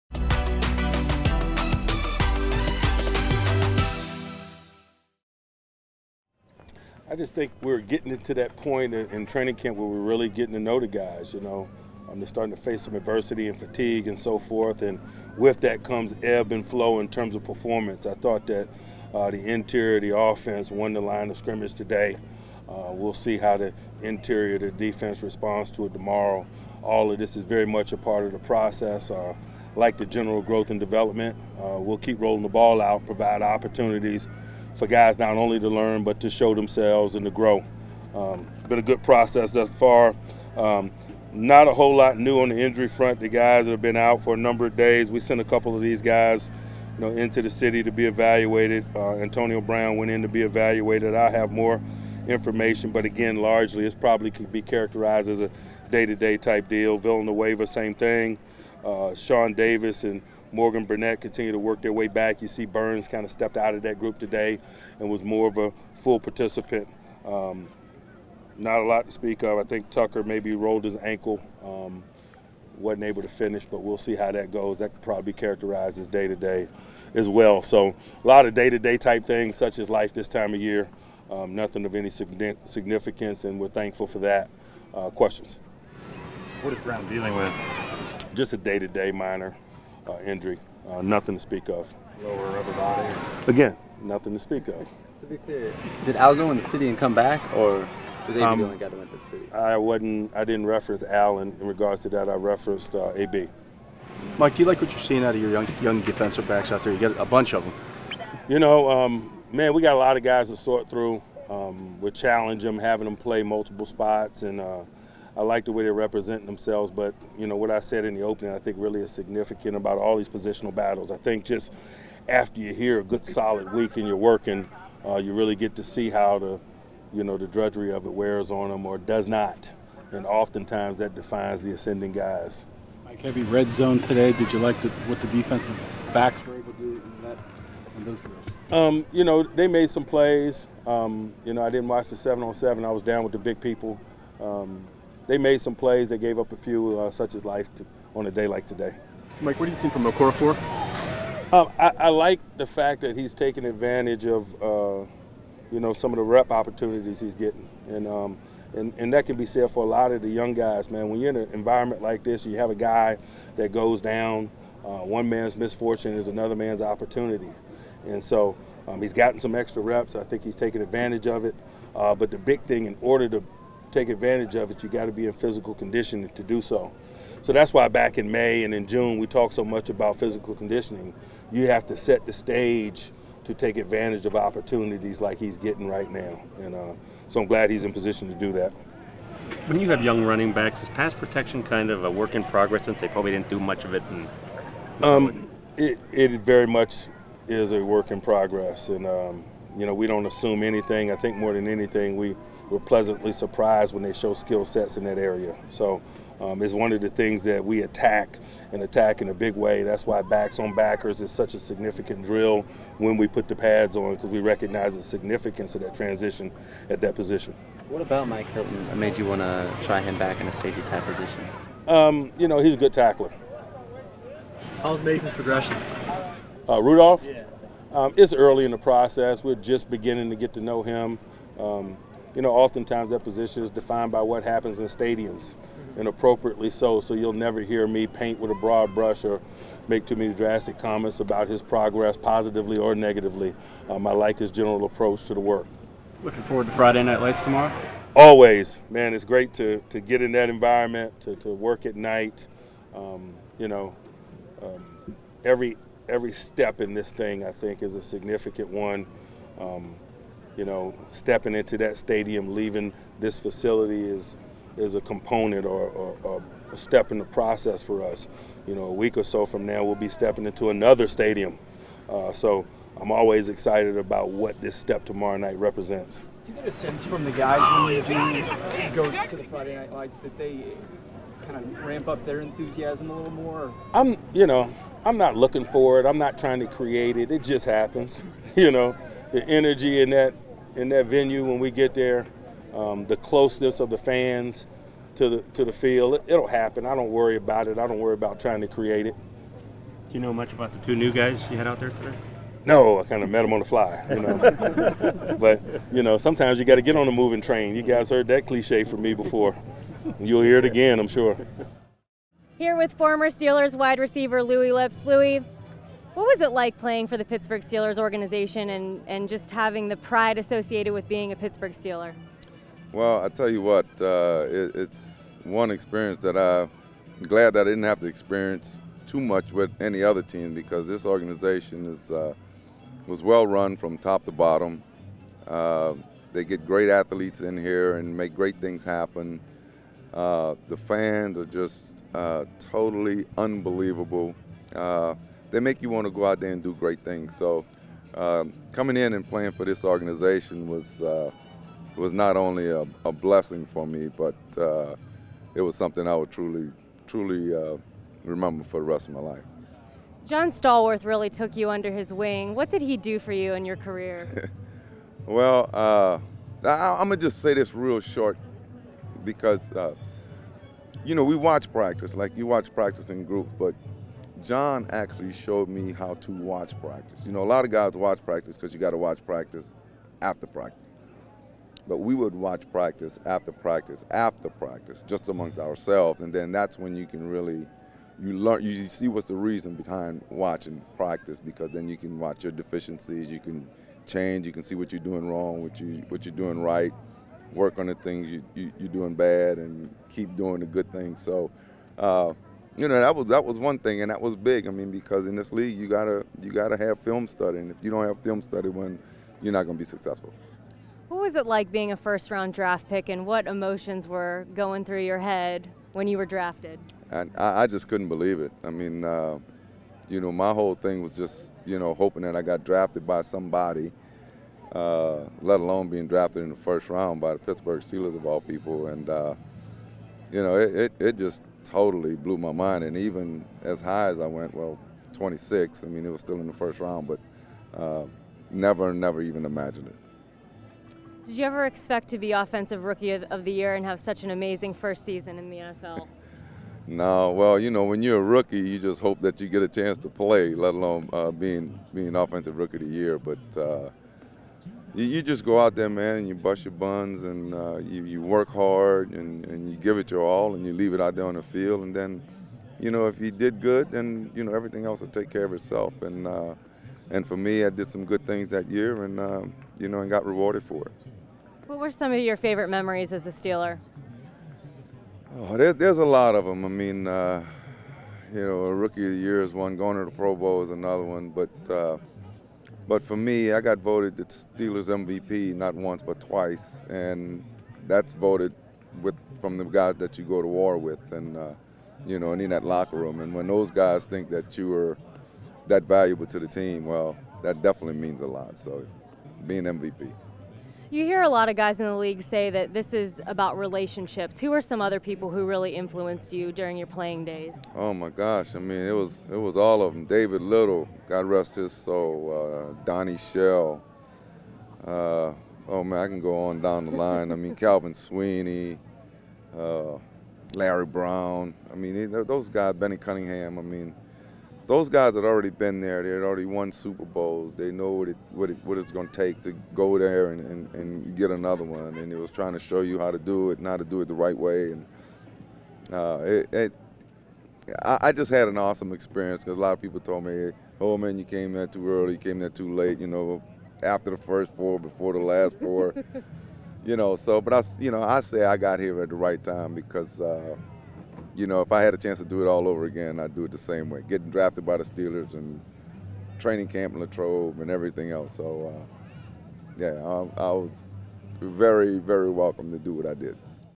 Coach Mike Tomlin, Louis Lipps recap Day 7 of Pittsburgh Steelers Training Camp
Pittsburgh Steelers Coach Mike Tomlin looks forward to how the defensive interior responds at training camp practice.

Former WR Louis Lipps reflects on his time in Pittsburgh.